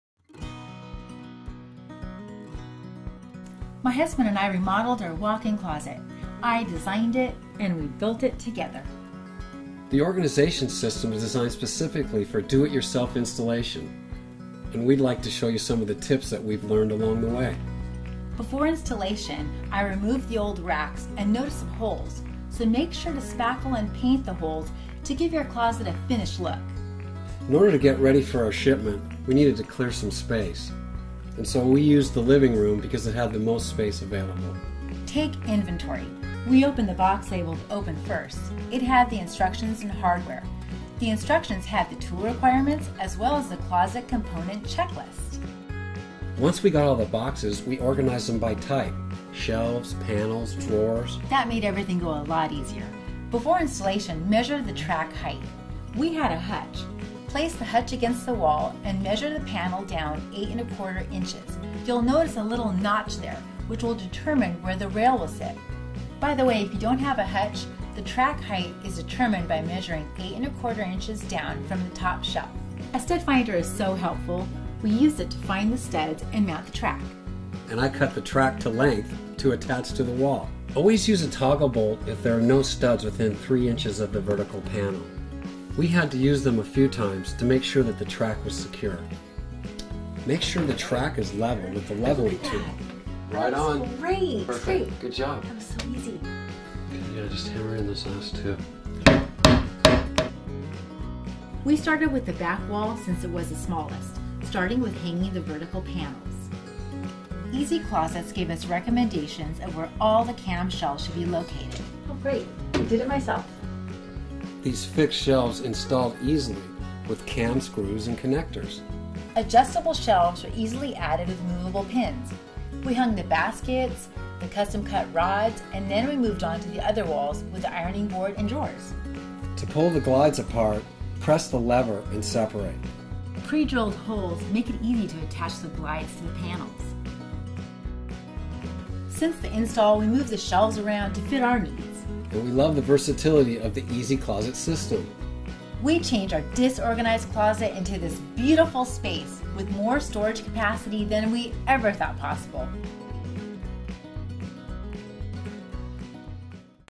Tips and Ideas for Installing Your Closet System
Use these tips when installing your own EasyClosets closet system for a project that's easy and a closet transformation that's amazing!